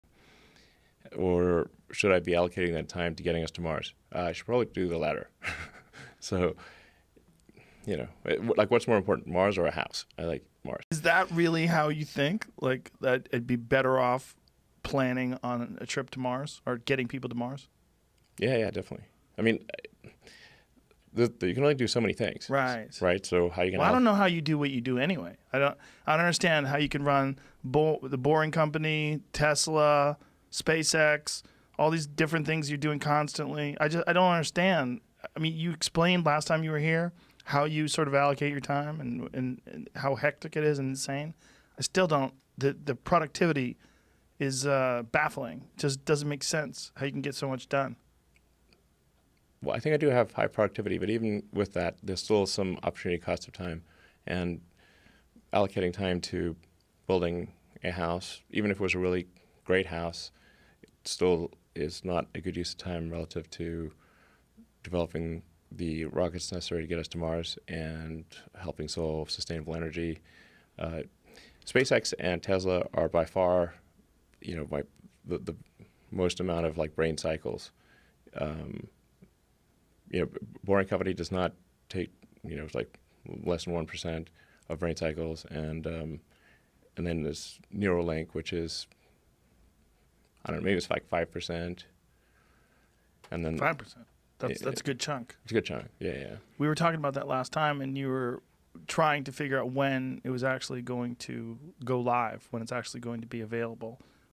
Mars Missions vs. Building Houses: Elon Musk's Priorities
Join us as we explore Elon Musk's perspective on prioritizing Mars missions over traditional investments like housing. Discover how he allocates his time between SpaceX, Tesla, and more while striving for sustainable energy solutions and our future in space.